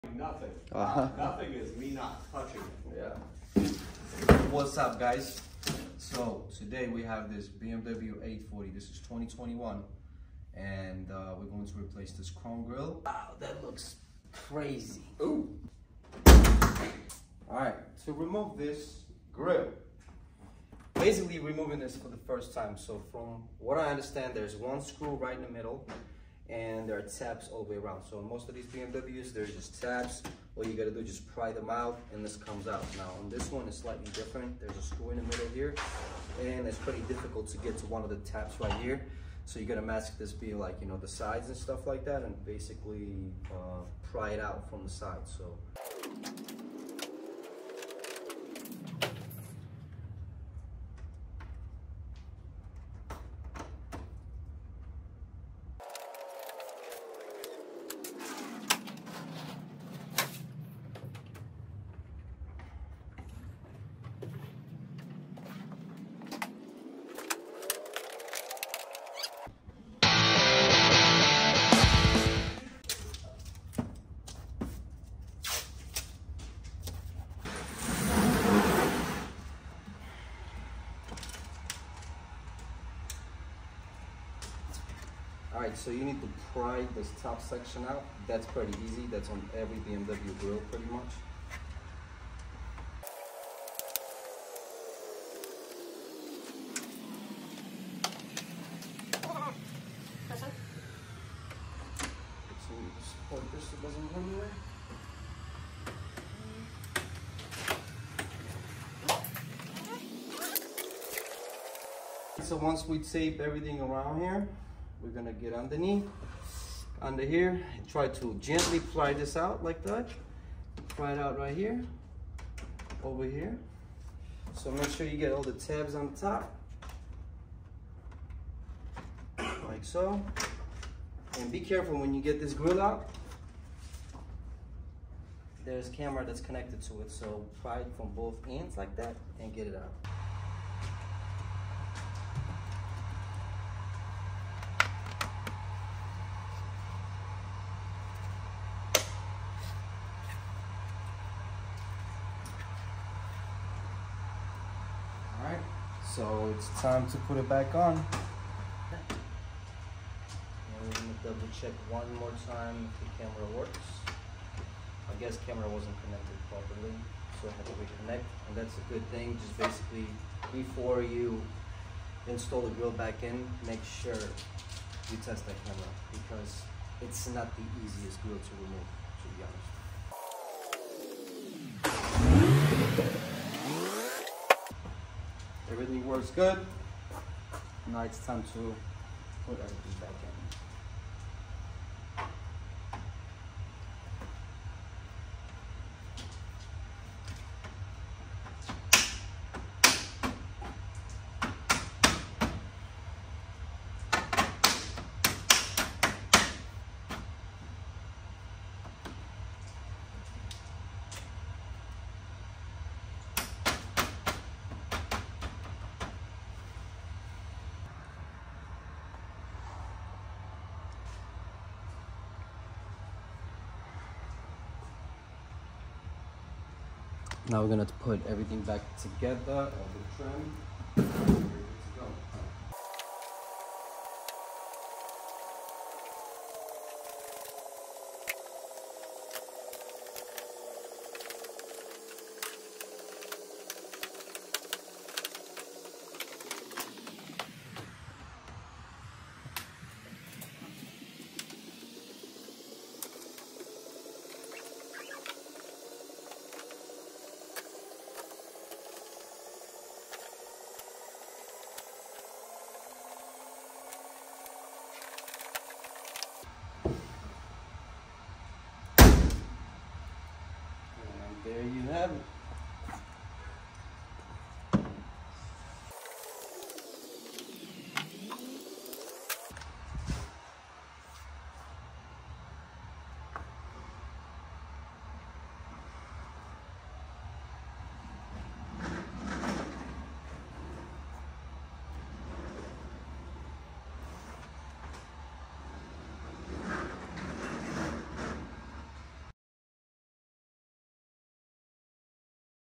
BMW 8 series grille replacement
Replace the grille on your BMW 840i, M8, or M850i to upgrade your vehicle’s look with this straightforward guide. Whether you’re doing a DIY modification or need to replace a damaged grille, this video covers all the essential steps. Perfect for BMW 8 Series enthusiasts looking to customize their cars, including M8 and M850i models. Get tips on the tools required, removal of the old grille, and proper installation of the new one to give your BMW a fresh, stylish appearance. Ideal for anyone looking to enhance their BMW's front-end aesthetics.